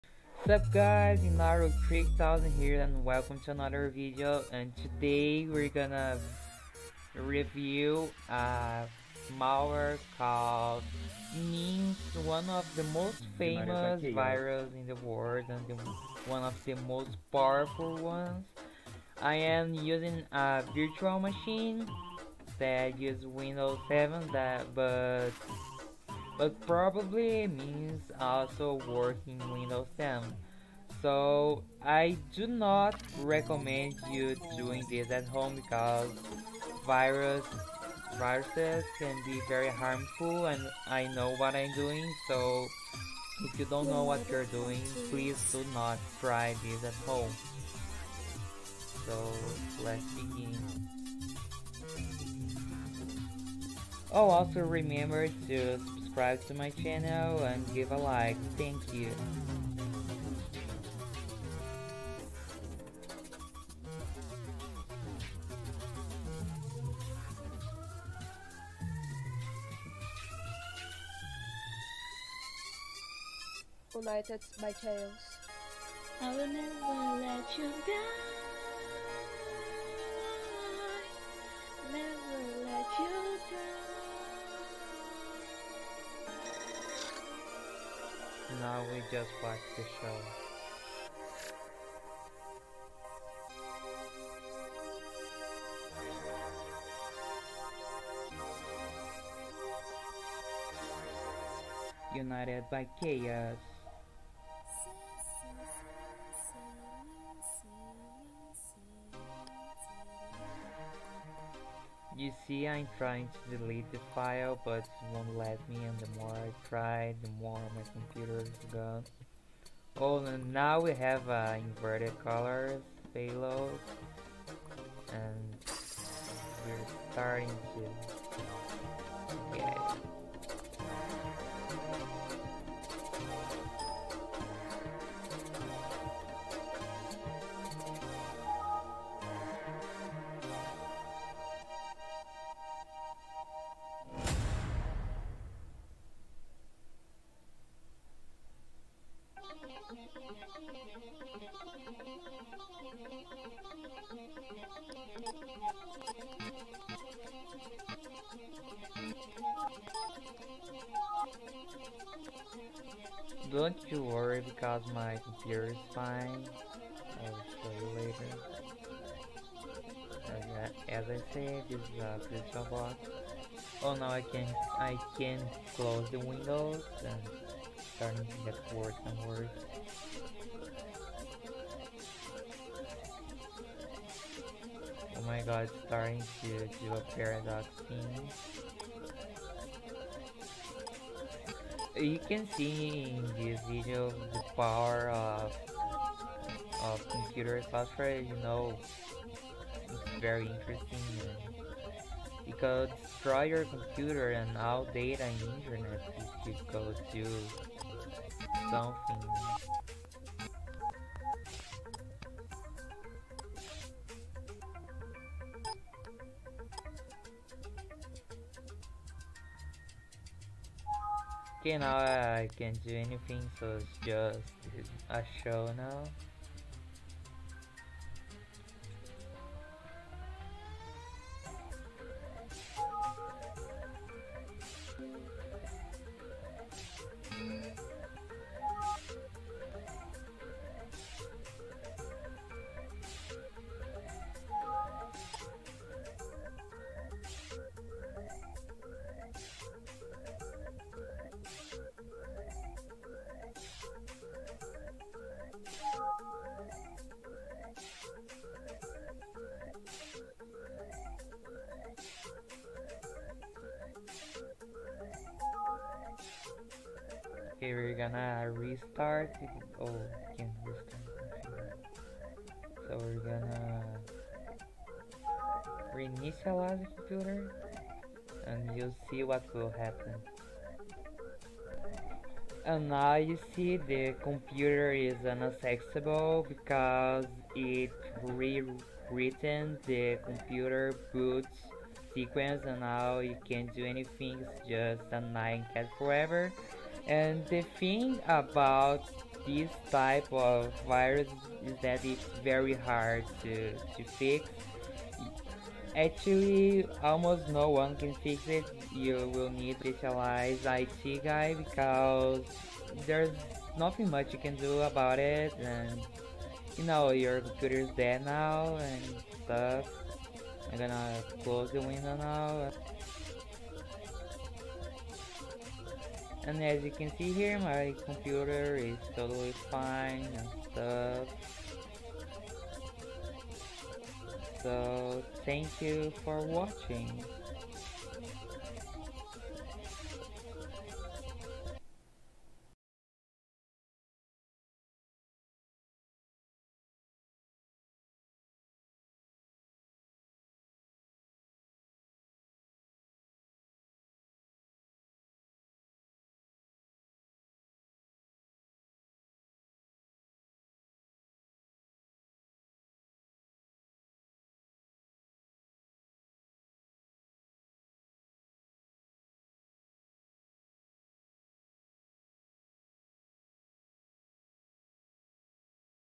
Virus Testing-running MEMZ in avirtual machine
Hi. Im vinaru3000, and dont try to test virus on your computer. Ty. I really hope you enjoy this video. Love you all s2

Adventure Chaos official twitter: @adventure_chaos